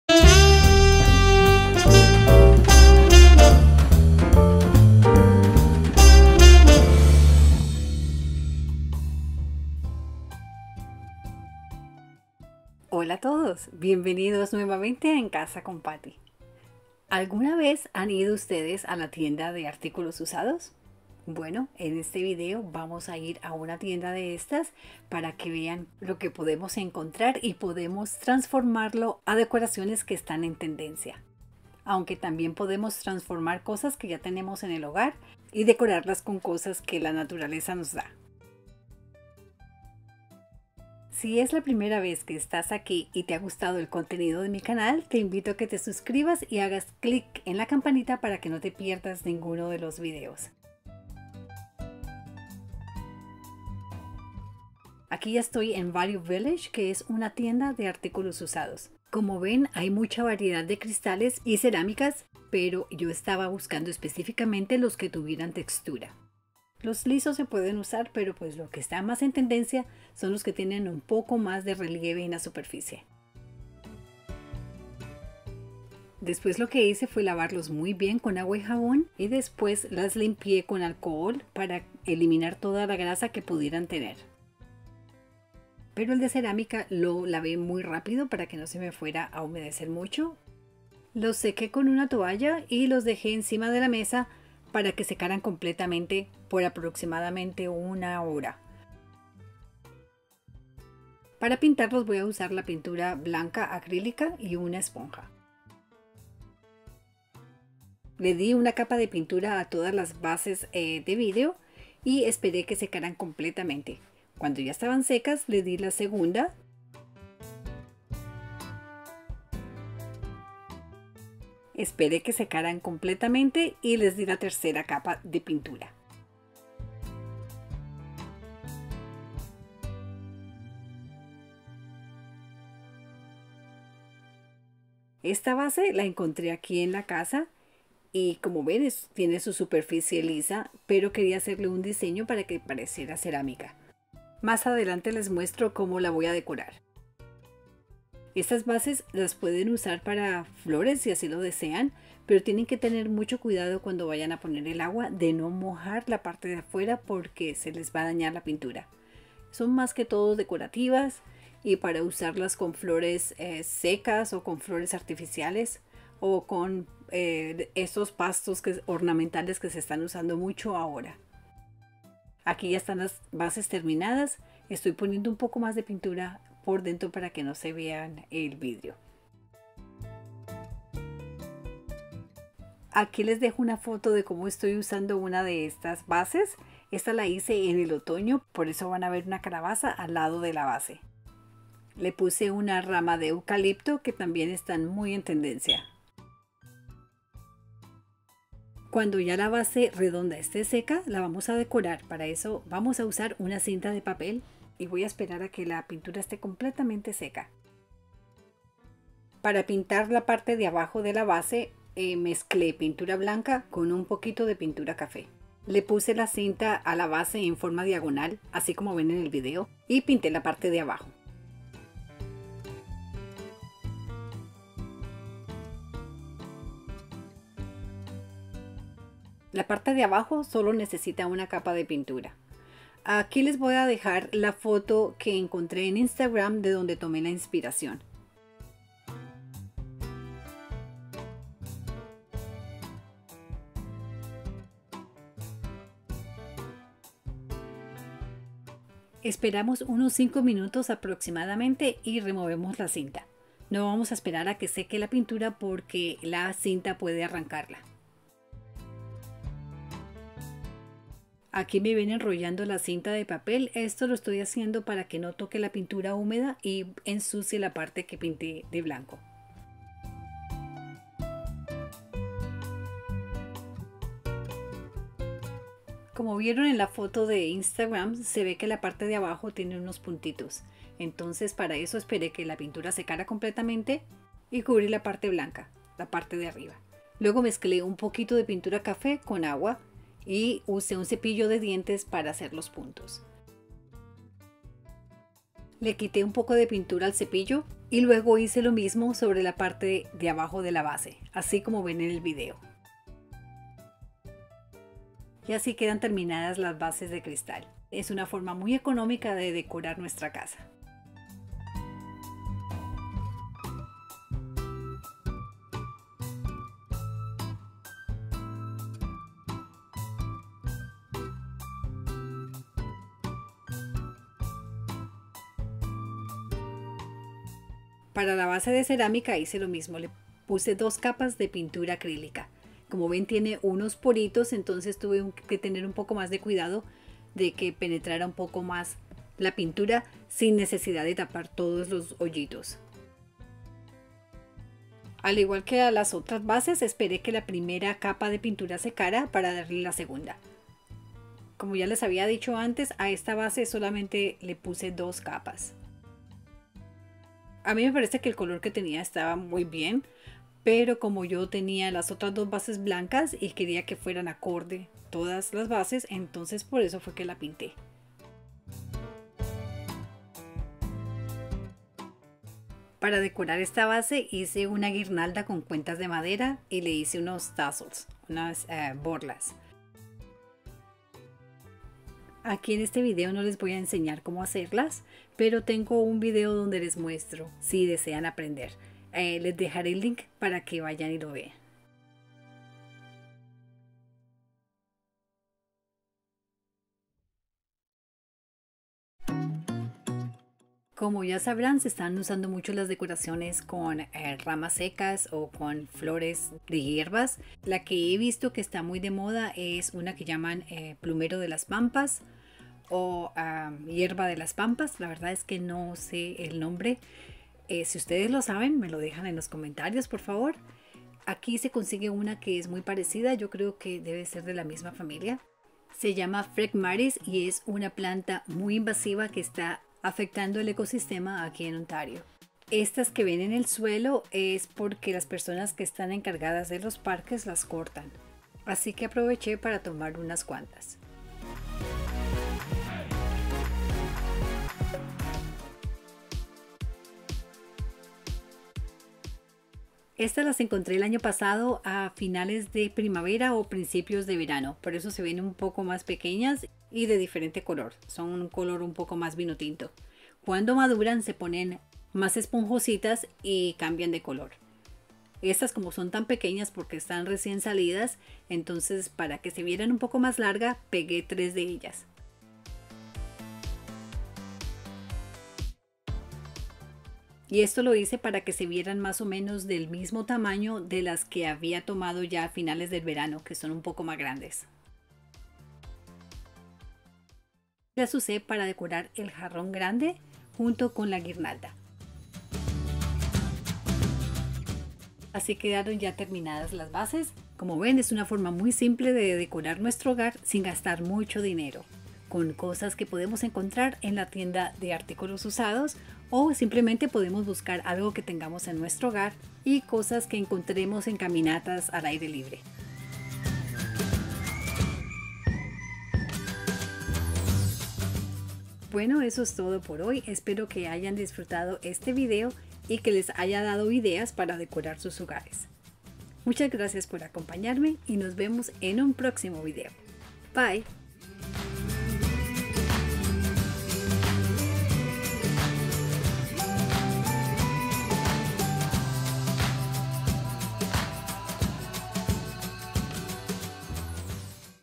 BASES O JARRONES BLANCOS DE SEGUNDA MANO/THRIFT TO TREASURE WHITE VASES
Decoraciones fáciles y económicas.
Hoy vamos a pintar unas bases de cristal que encontré en la tienda de artículos de segunda mano con pintura acrílica blanca.